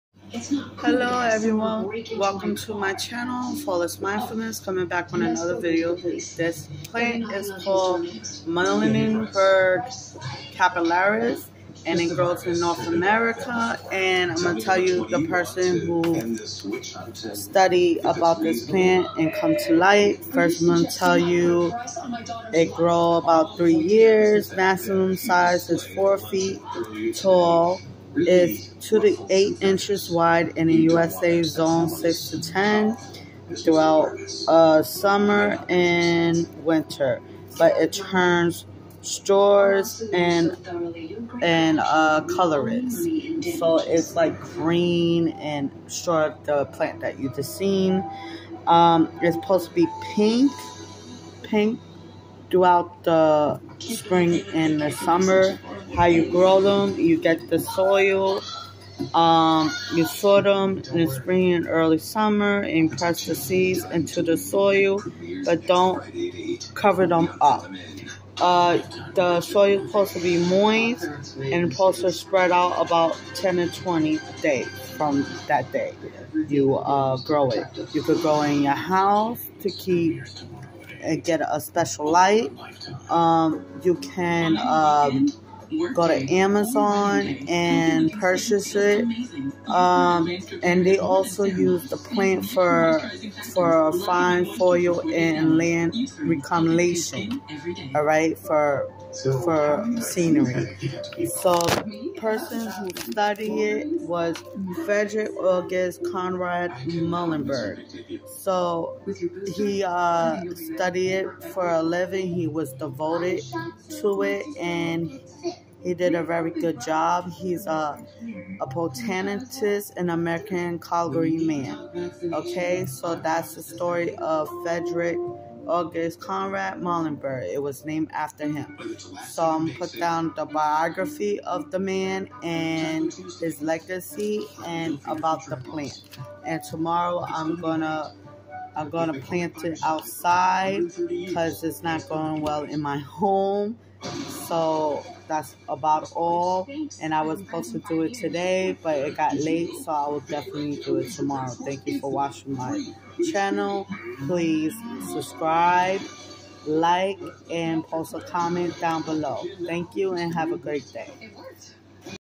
Muhlenbergia Capillaries
muhlenbergia capillaris, commonly known as the hairawn muhly, is a perennial sedge-like plant that grows to be about 30–90 cm (0.98–2.95 ft) tall and 60–90 cm (2.0–3.0 ft) wide. The plant includes a double layer; green, leaf-like structures surround the understory, and purple-pink flowers outgrow them from the bottom up. The plant is a warm-season grass, meaning that leaves begin growth in the summer. During the summer, the leaves stay green, but they morph during the fall to produce a more copper color

How to grow the pink grass
Muhly grass seed germination requires light, so press the seeds down as you scatter them but don't cover them in a layer of soil or compost. Keep the seeds moist until they sprout and grow into seedlings. You can grow muhly grass from seed by starting indoors, which helps to keep the seeds warm enough.
the grass is use fine ⛽ Fuel

where found Muhlenbergia Capillaries  through our north America

The Founder is a American Clergyman and Botanist  his name is Frederick Augusta Conrad Muhlenburg if you want to know more information please  look up his Biography.

you found this pink grass on Amazon or your local garden store 🏡 .

And tomorrow I plant the grass in front of my 🏢 building complex.